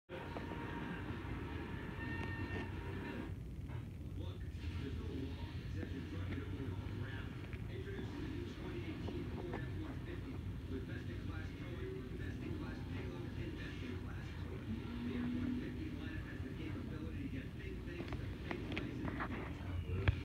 My Message Read Description
I am taking a 2 month break because I am very stressed right now and it’s hard to edit By the way let’s Hit 10 subs while I’m gone!